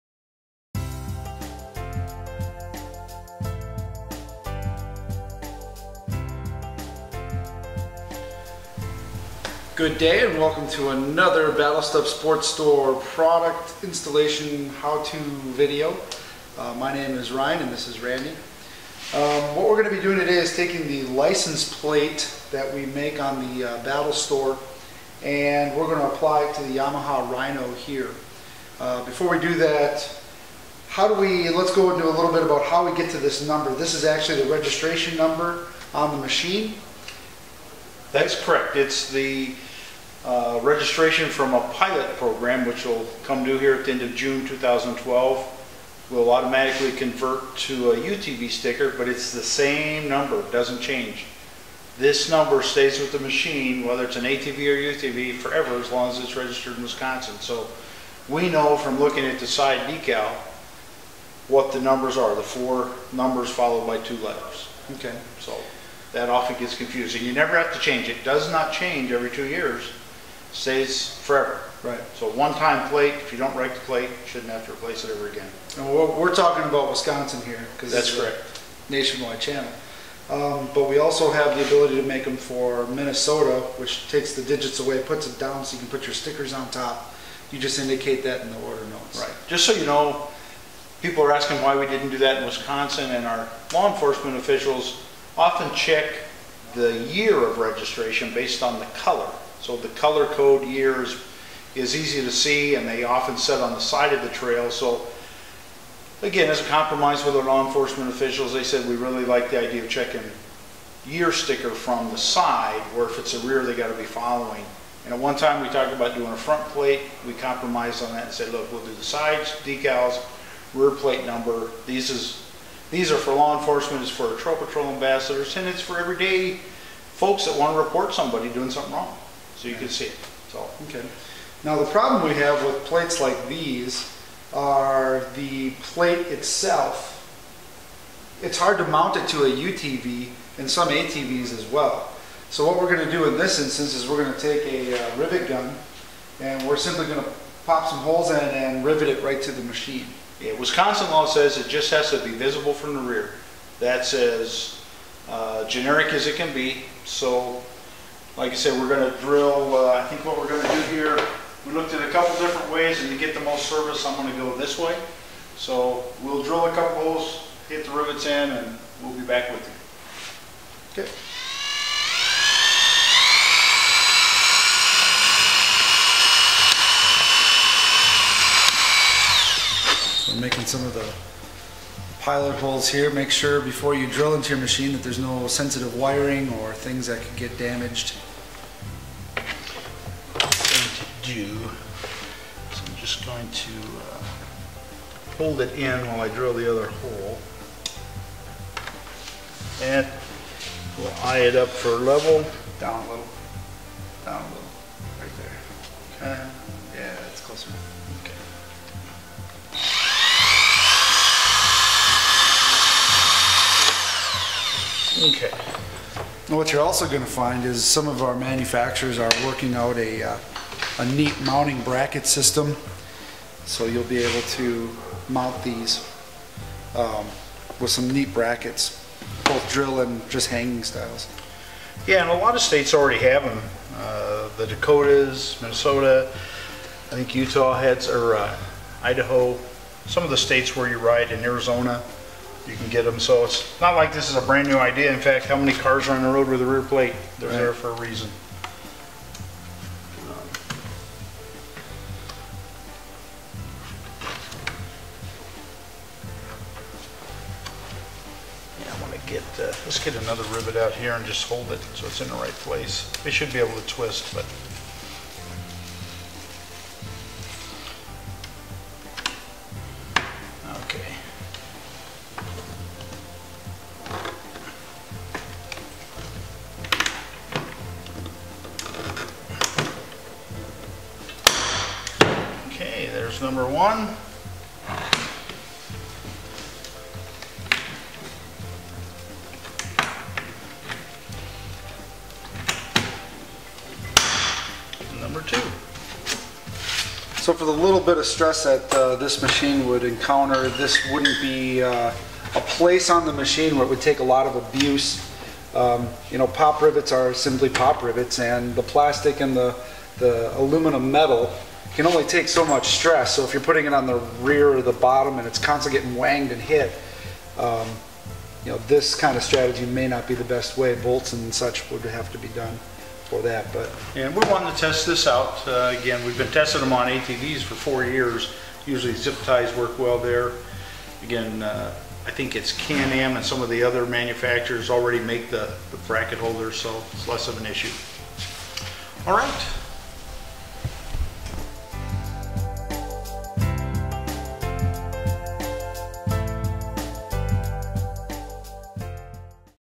Plate install video
A simply video showing how we tossed a plate on a Rhino as well as explaining the license plate process a little bit.